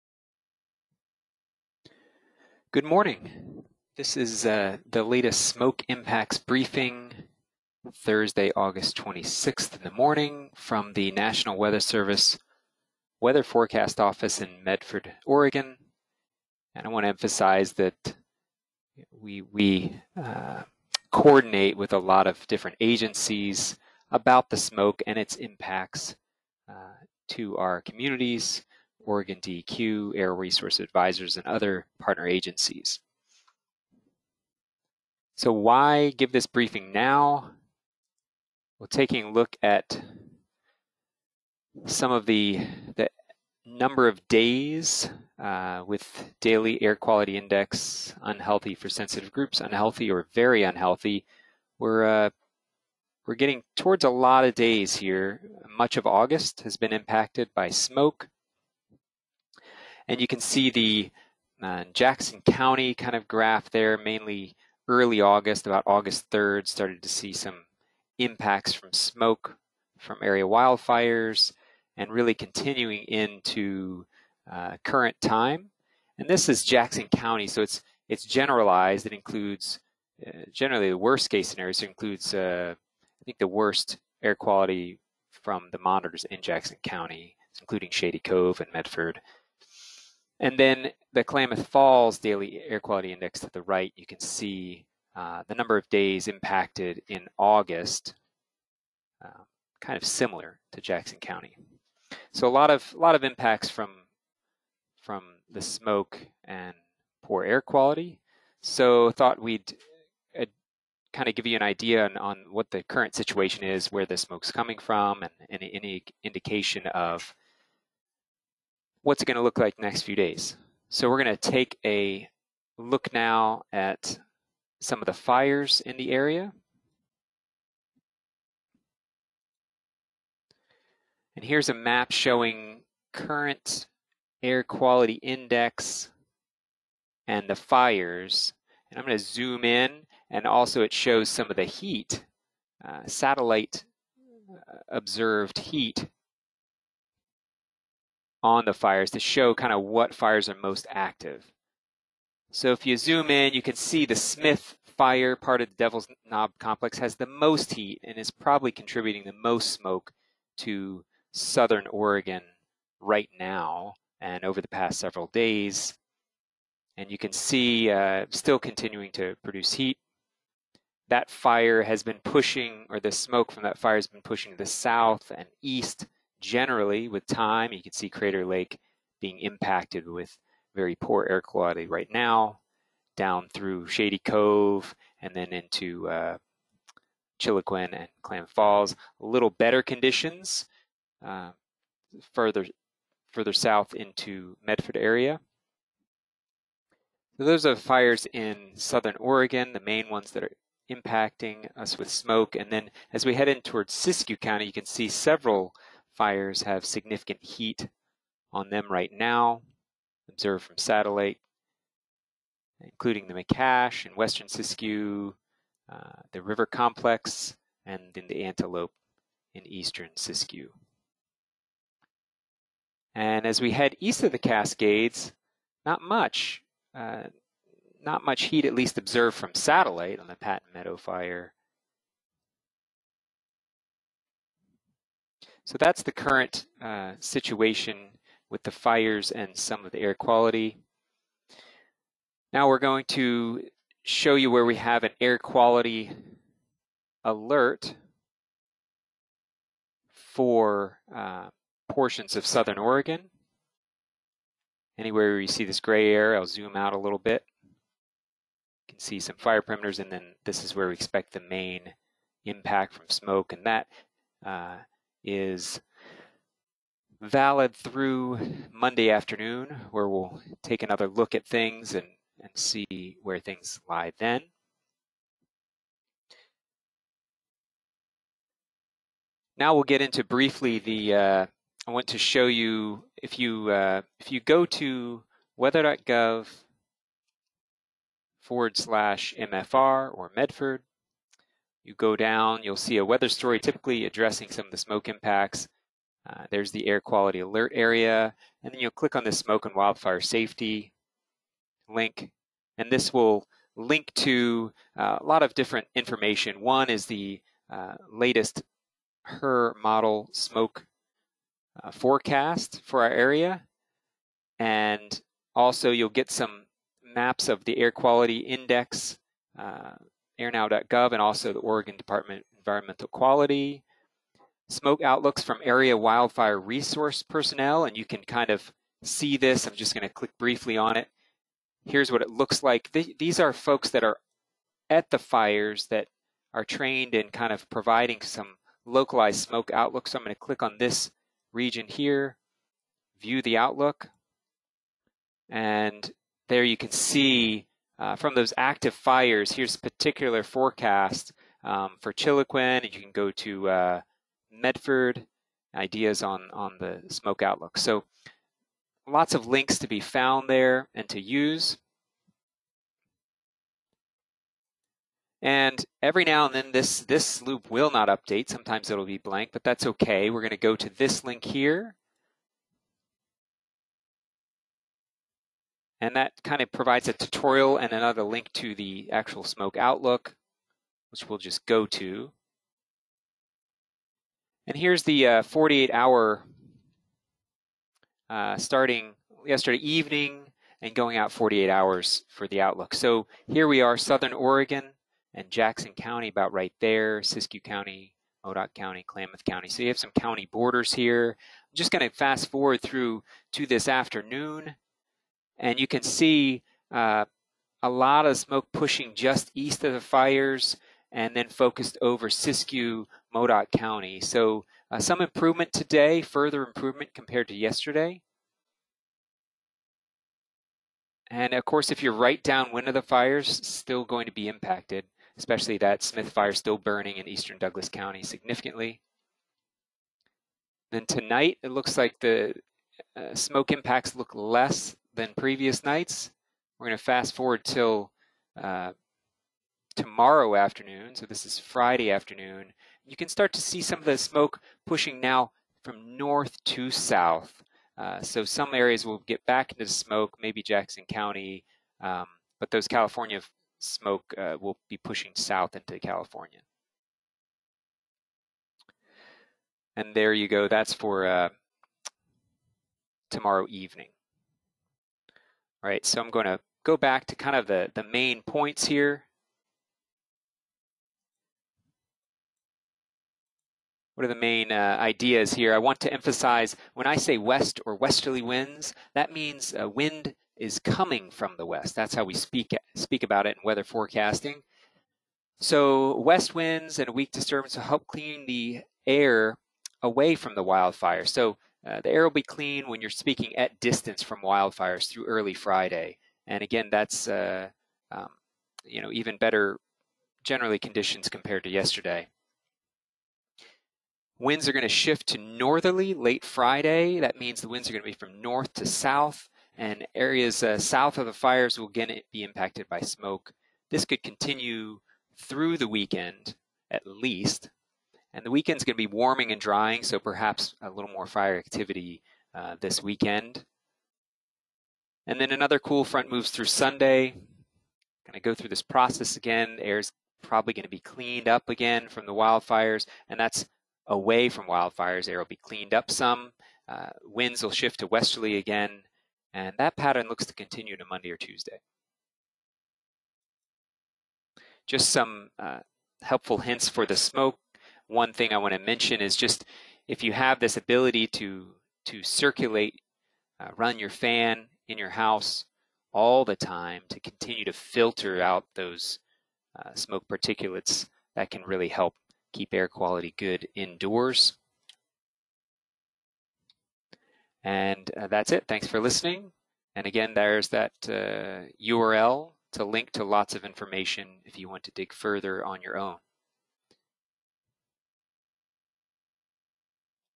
Smoke Impacts Outlook for Southern Oregon and Northern California
Current wildfire and smoke situation for Medford NWS forecast area, with an outlook into next week.